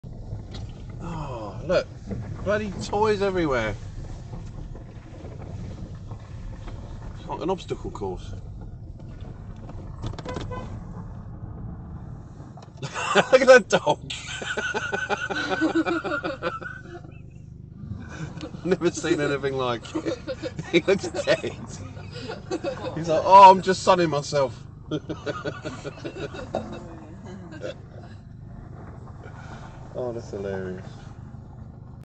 Taxi Antics: Dog looks dead sunbathing in traveller site - It's a dog's life! Who'd be a dog?
Obstacle course carefully laid out for me to negotiate on a daily basis.